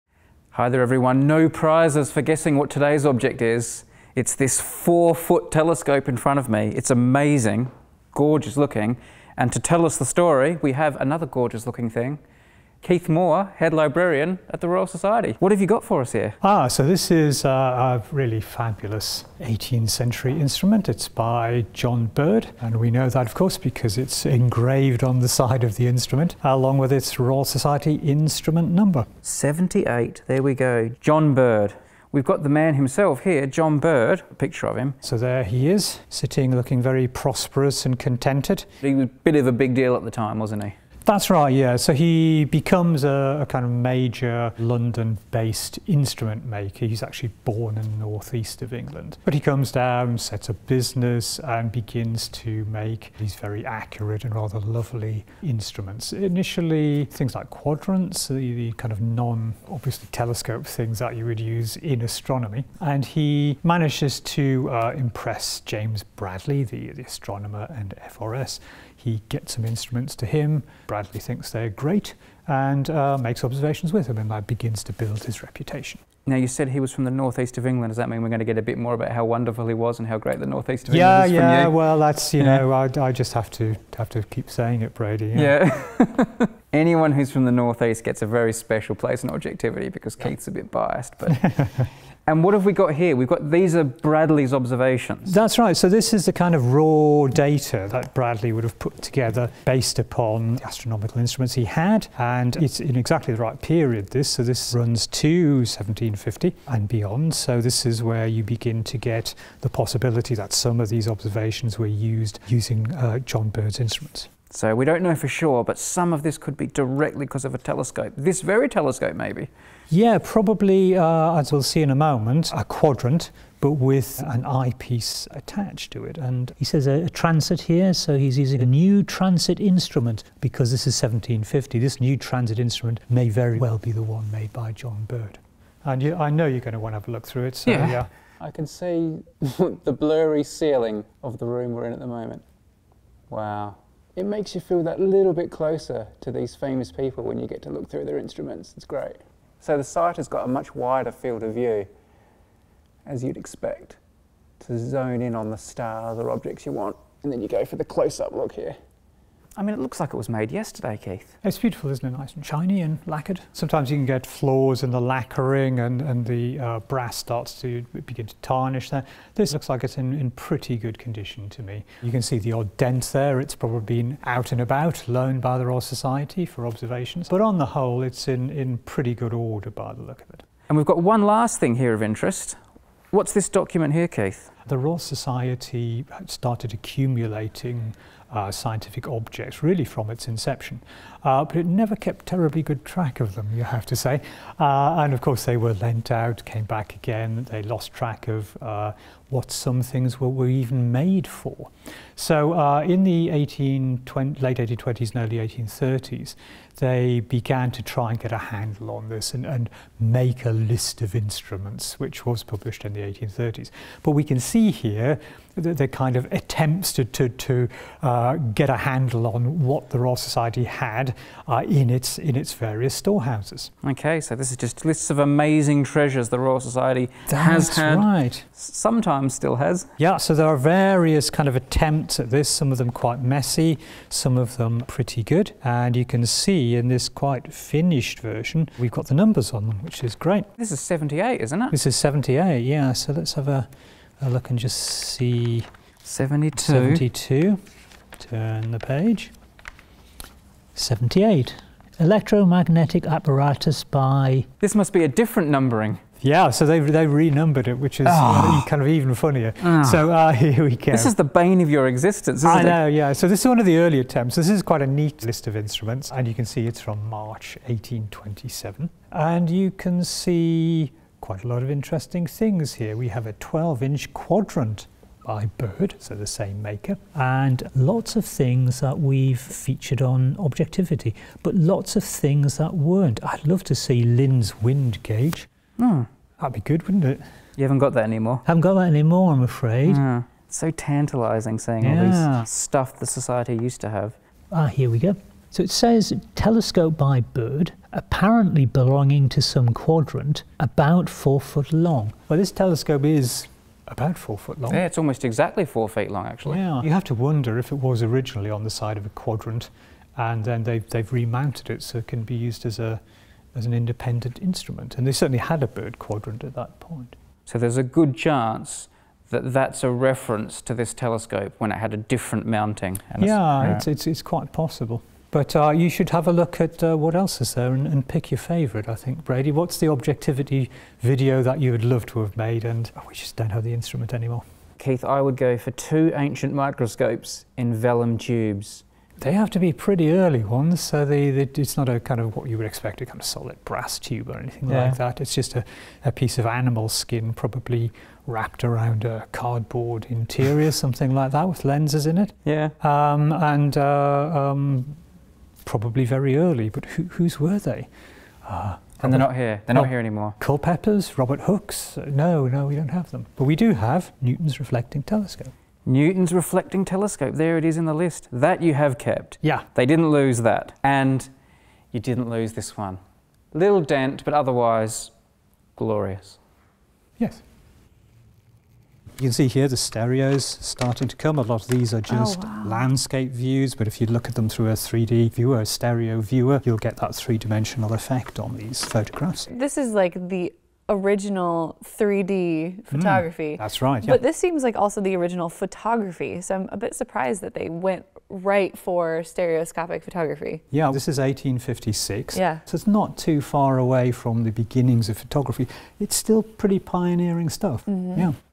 Four Foot Telescope - Objectivity 252
We look at a beautiful 18th Century telescope at the Royal Society. More links below ↓↓↓

Some keywords: Telescope, four foot, John Bird, instrument maker, quadrant, James Bradley, observations, instrument list, microscopes, vellum, Newton's reflecting telescope,